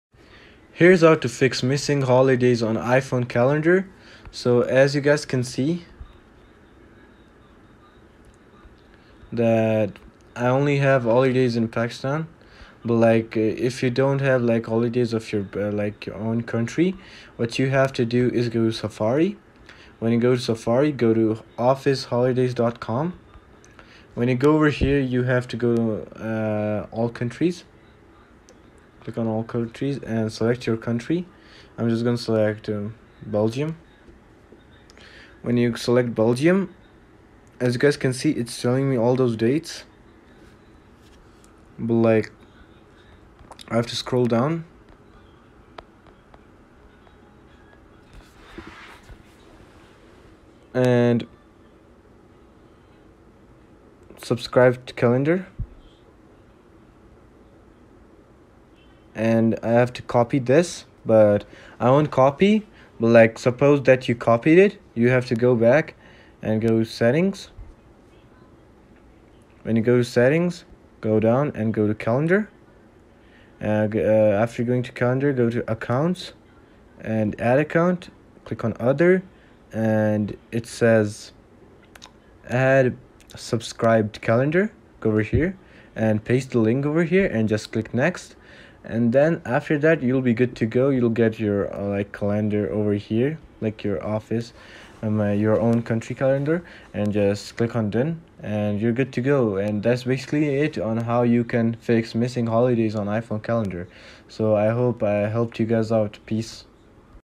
How to FIX Missing Holidays on iPhone Calendar
If you are looking for a video about How to FIX Missing Holidays on iPhone Calendar, here it is!
In this video I will show you how to add holidays in iphone calendar. Be sure to watch the video to the very end.
You will learn how to change calendar holidays on iphone.